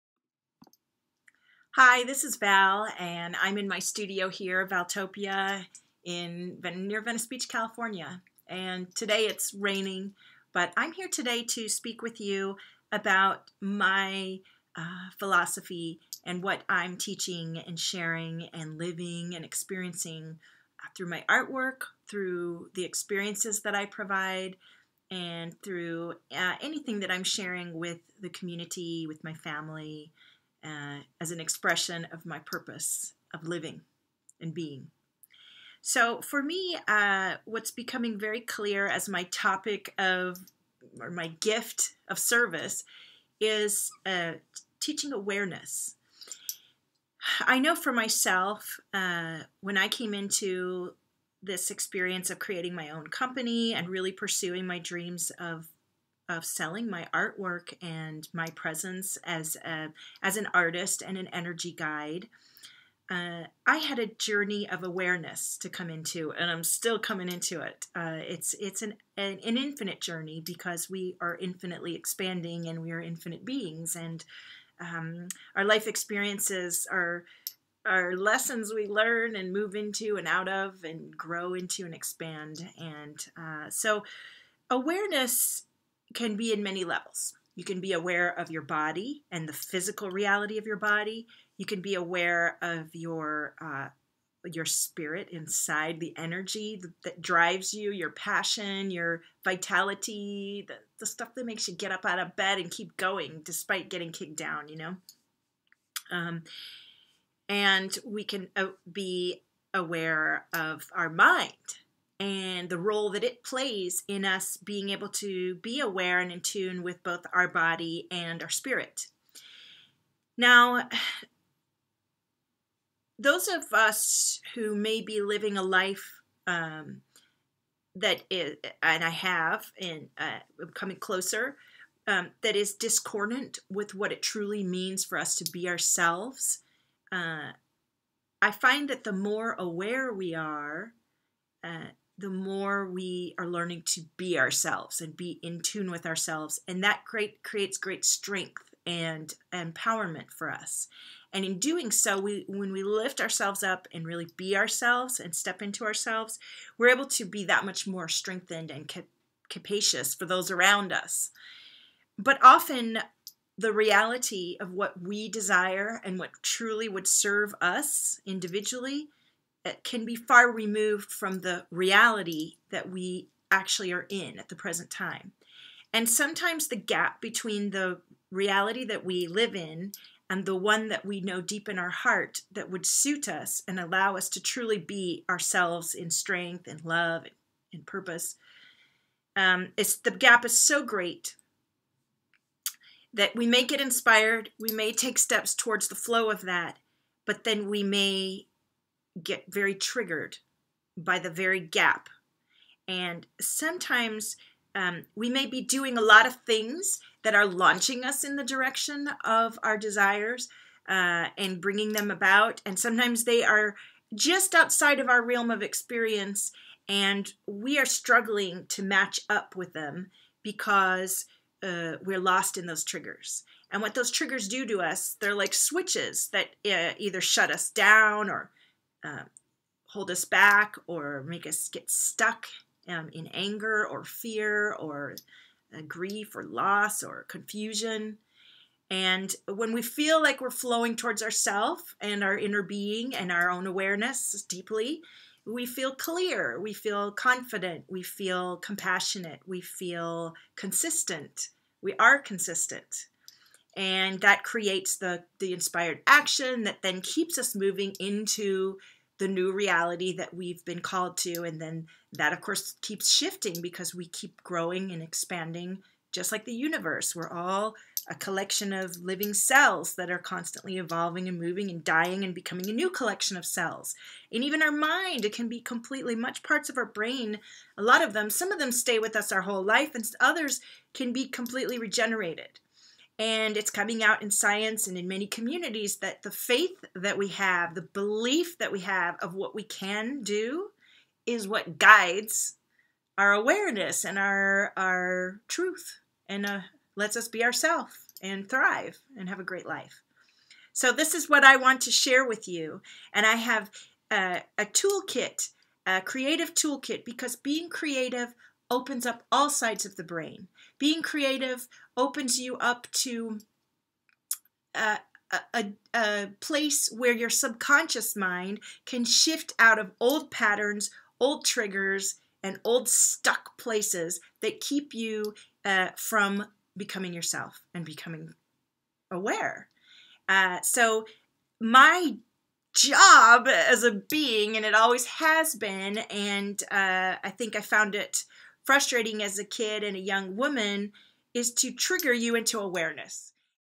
Valtopia Awareness
How energy and self awareness leads you into your flow and joy.